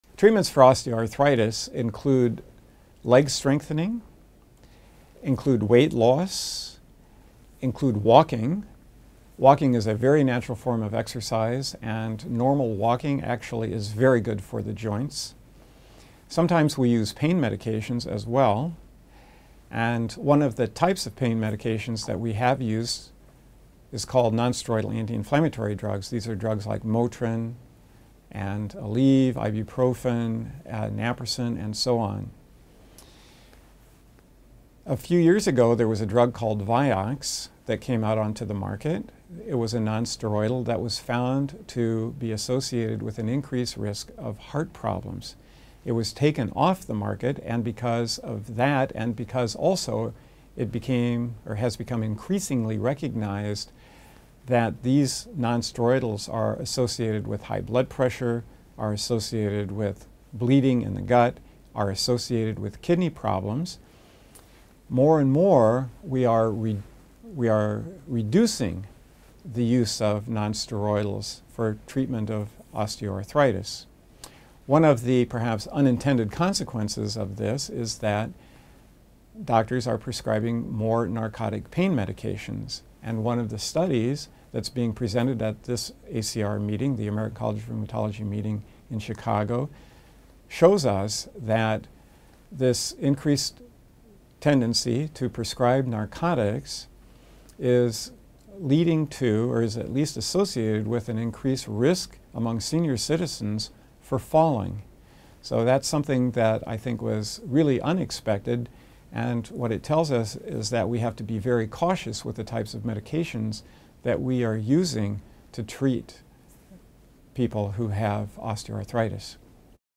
Unintended consequences of latest osteoarthritis treatments
Experts on rheumatoid arthritis, gout and other diseases are gathering at the American College of Rheumatology's annual scientific meeting in Chicago. Eric Matteson, M.D., chairman of rheumatology at Mayo Clinic in Rochester, Minn., talks about the latest in research and treatment.